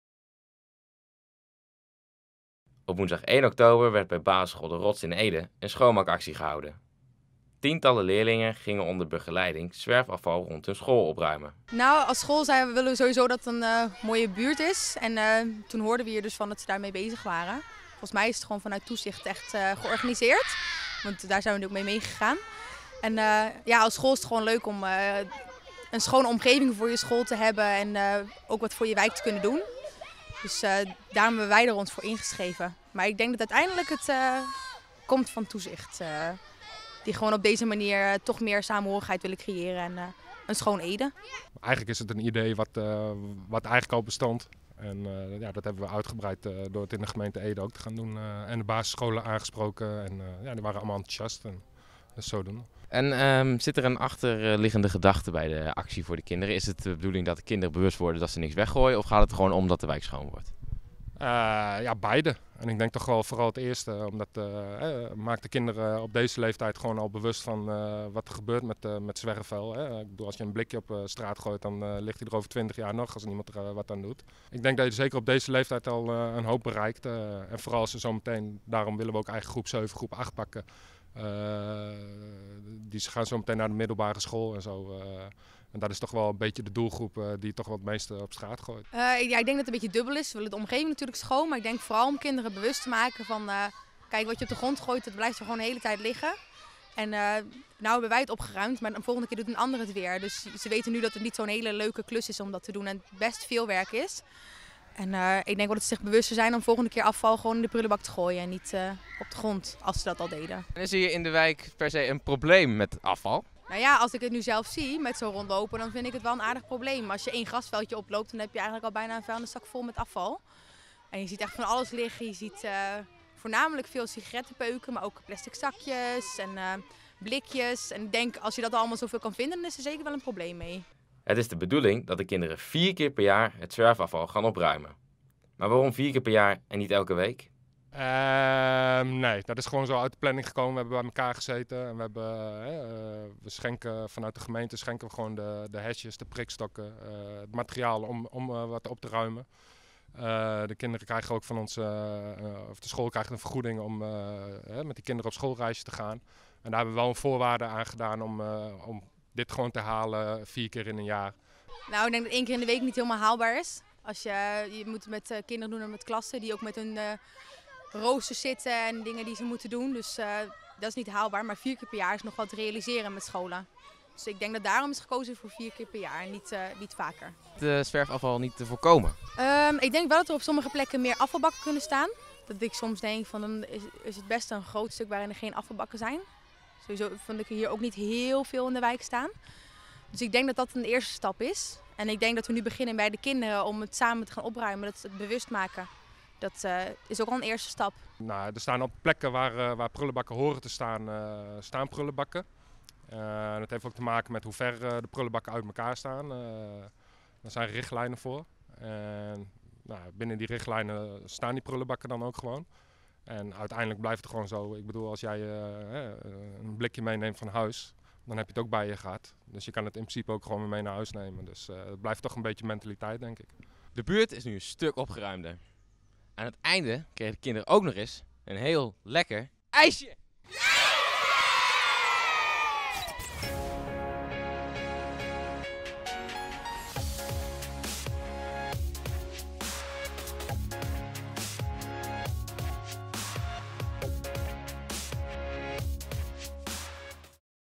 Leerlingen De Rots steken de handen uit de mouwen
EDE - Leerlingen van basisschool De Rots in Veldhuizen staken woensdagmorgen hun handen flink uit de mouwen. Tientallen leerlingen van de school deden mee aan Ede Schoon, een opruimactie tegen zwerfafval.

-- Helaas is er in de montage van deze video een fout geslopen. In de titelbalk wordt gesproken over basisschool De Korst, dit moet zijn De Rots --